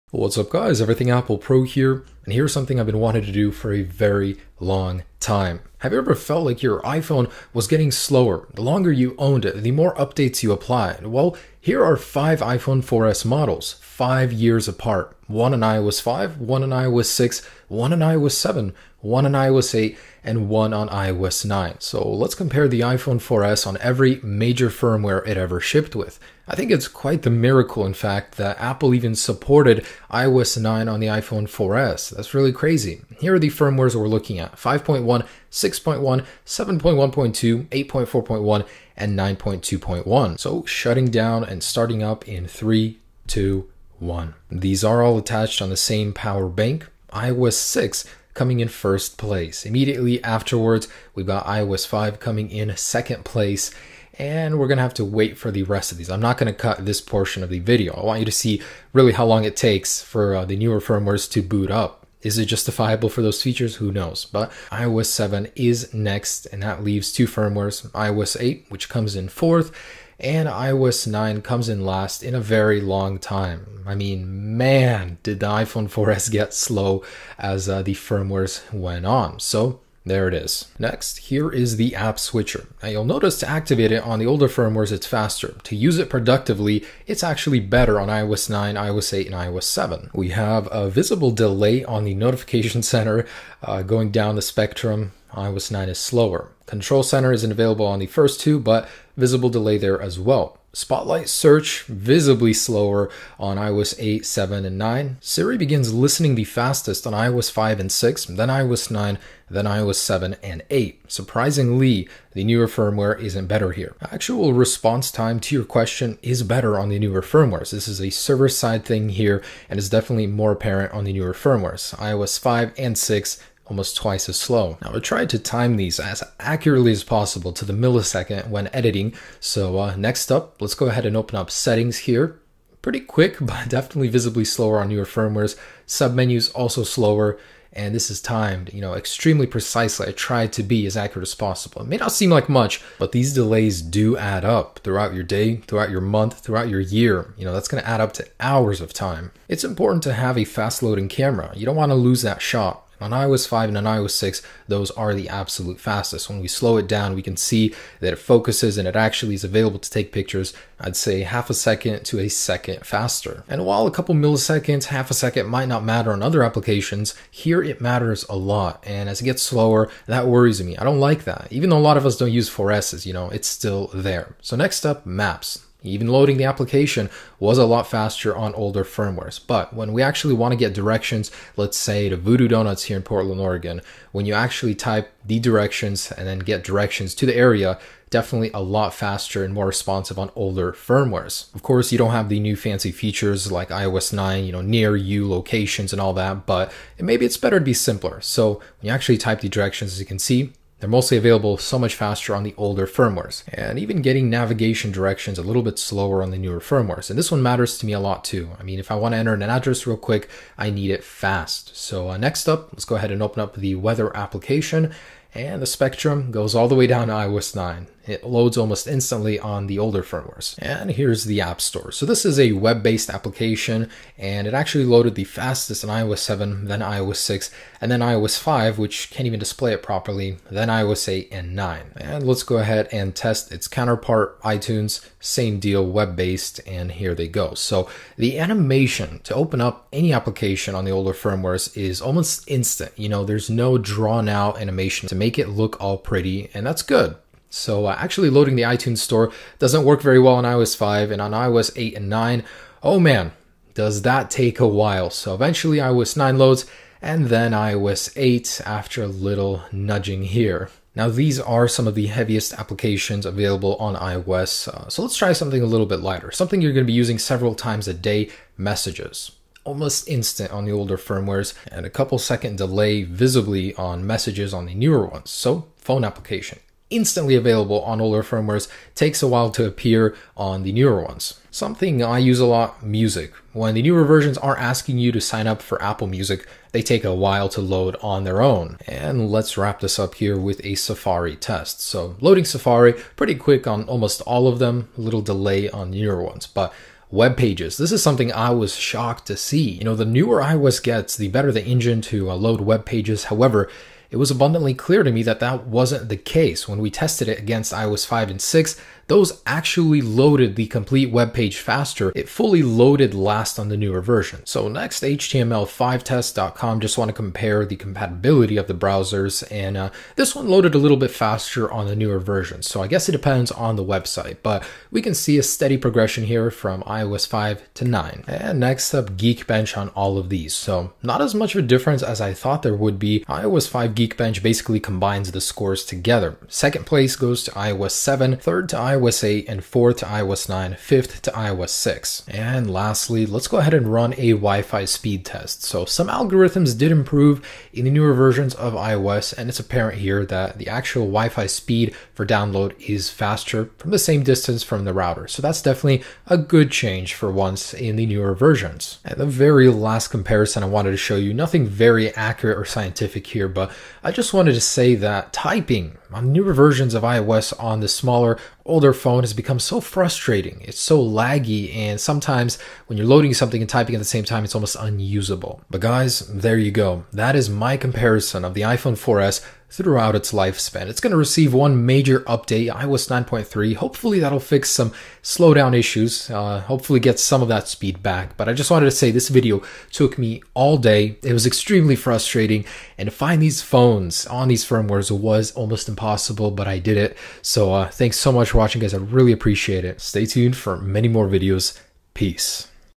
iOS 5 vs iOS 6 vs iOS 7 vs iOS 8 vs iOS 9 on iPhone 4S Speed Test
iPhone 4S on EVERY Firmware Speed Test! Planned Obsolescence Real? iOS 5 vs iOS 6 vs iOS 7 vs iOS 8 vs iOS 9 Comparison on 4S!